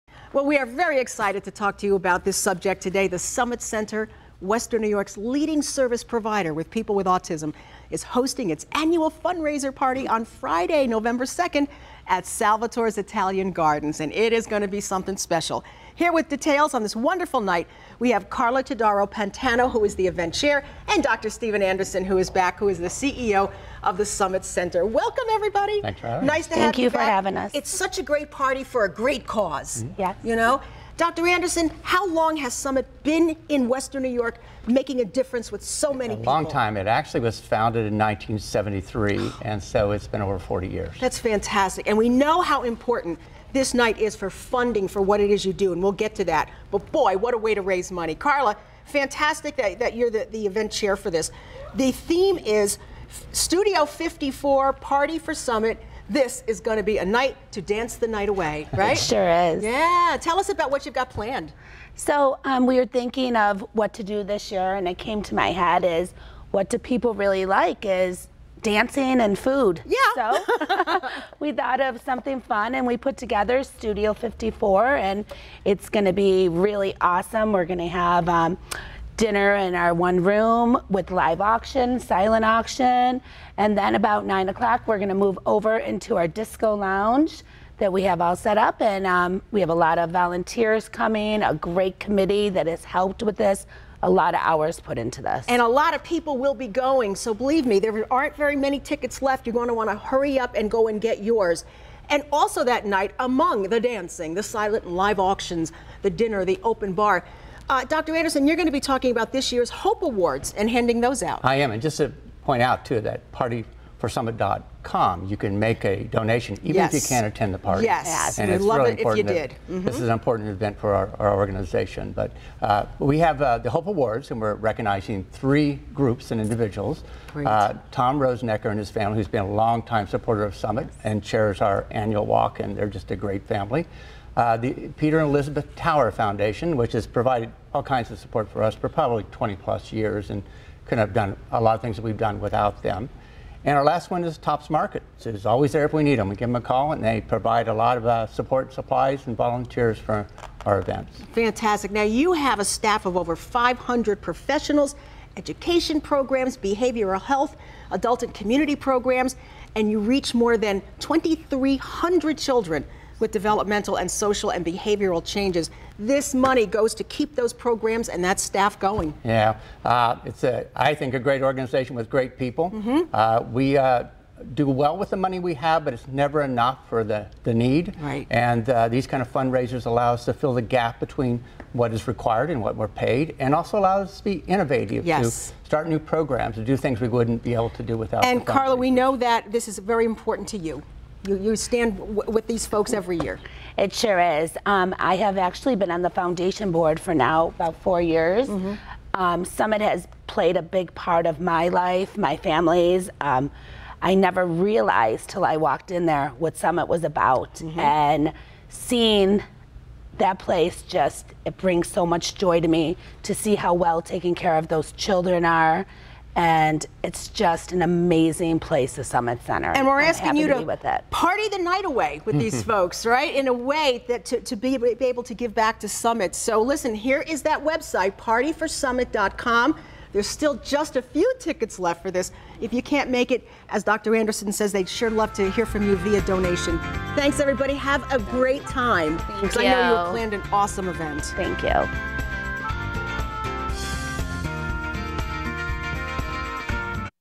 Party for Summit
The Summit Center, Western New York’s leading service provider for people with autism, will hosts its annual fundraiser, Party for Summit on Friday, November 2nd from 6 – 11pm.  It is taking place at Salvatore’s Italian Gardens.  The event theme is "Summit 54," and will feature a high energy DJ, LED dance floor, and the glitz of New York's famed Studio 54. There will be plenty of surprises in store along with great food, open bar and a number of silent and live auction items including a Backyard Party for 50, Sabres Suite for 18, and a Disney trip among others.  Proceeds will benefit Summit Foundation which supports The Summit Center, Western New York’s leading provider of evidence based educational, behavioral health, adult and community programs to more than 2,300 children and adults each year with developmental, social and behavioral challenges.